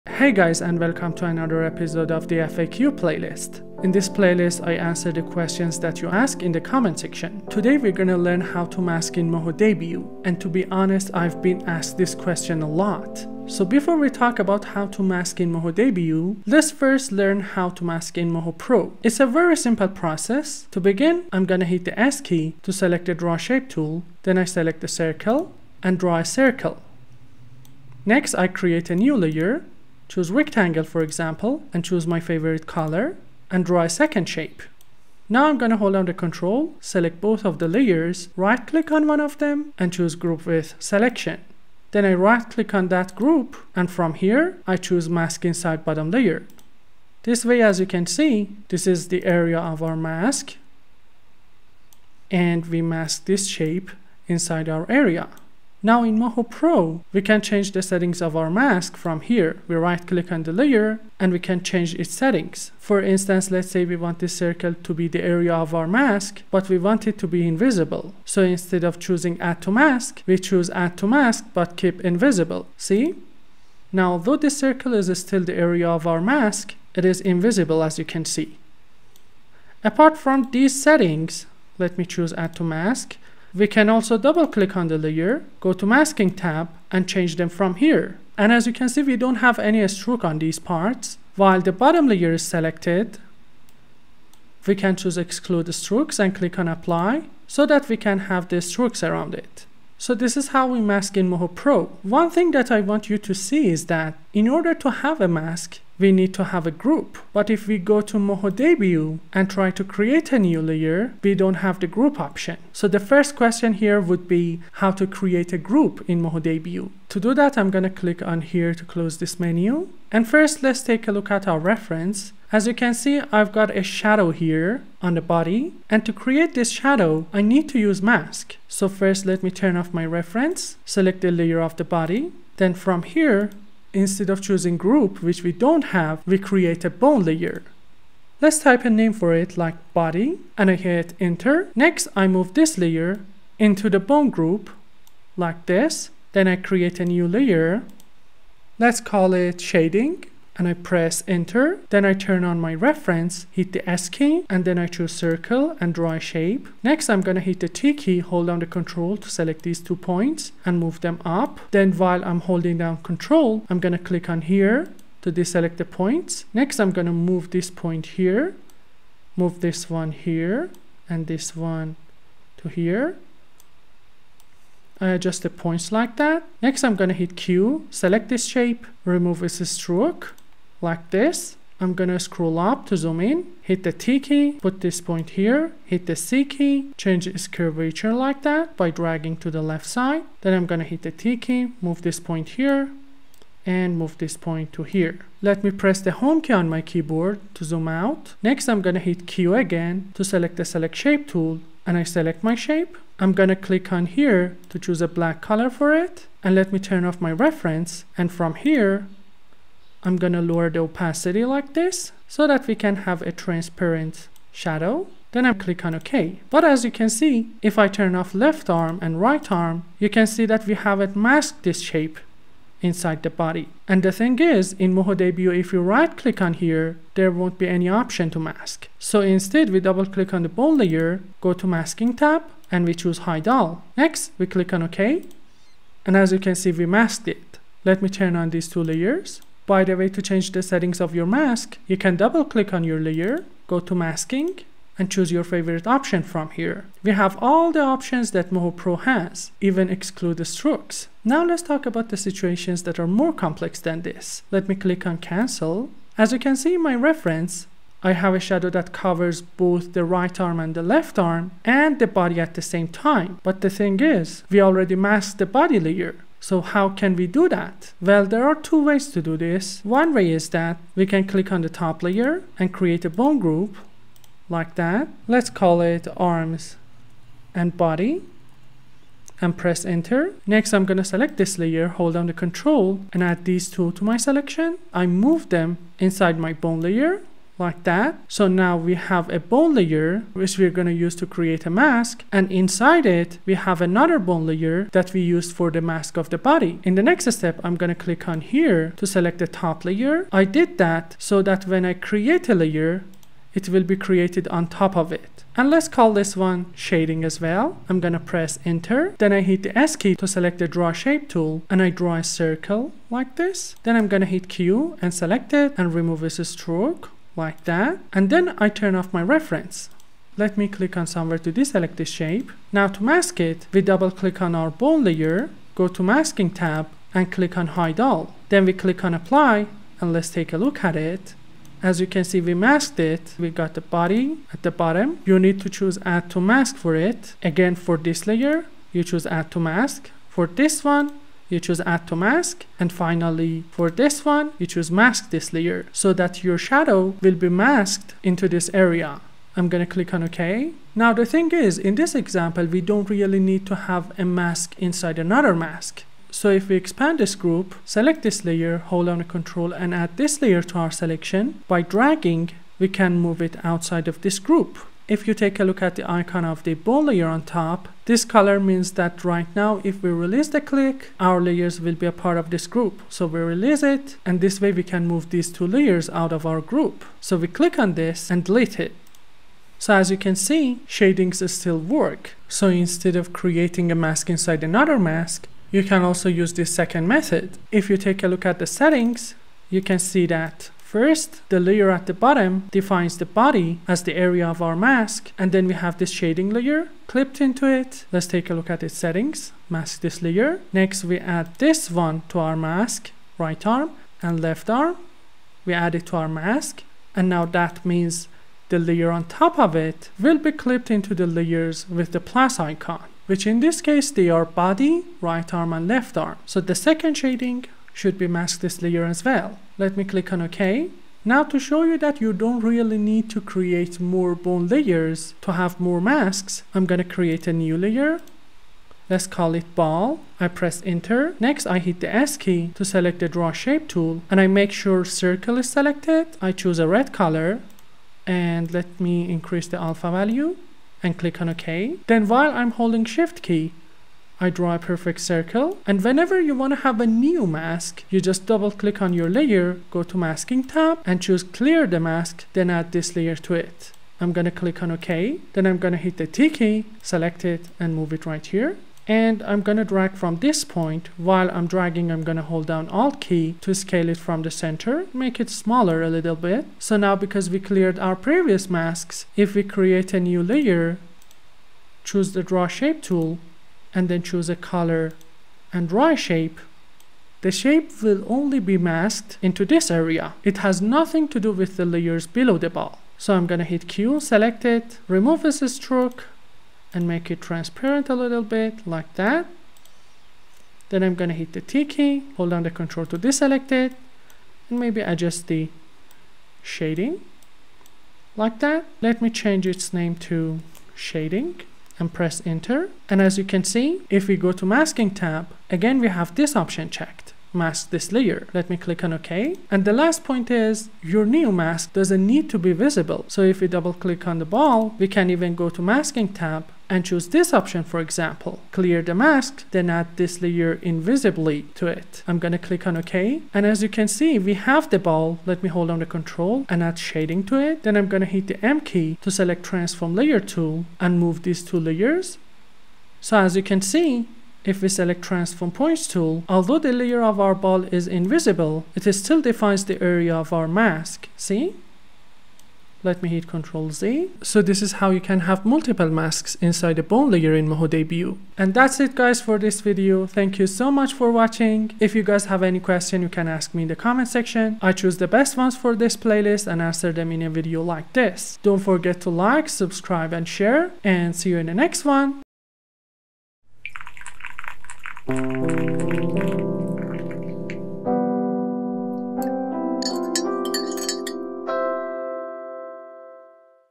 How to Mask in Moho Debut | Moho Debut 14.3
This video teaches you how to master masking in Moho Debut! You'll learn how to create groups, set up mask layers, and build masks within masks for more complex effects. We’ll also explore how to combine multiple masks, layer them together, and even create multiple masks inside one group to manage them efficiently.

Whether you're new to Moho Debut or looking to level up your animation techniques, this guide has you covered! I hope you'll enjoy this video and learn something new.

→ FAQ Episode 09

0:00 - Intro
0:13 - Mask in Moho Pro
1:52 - Creating a group in Moho Debut
3:36 - Mask in Moho Debut
3:59 - Change Mask settings
4:15 - Mask inside another mask
5:50 - Adding Multiple Layers to mask
7:00 - Having multiple masks in one group
9:08 - Outro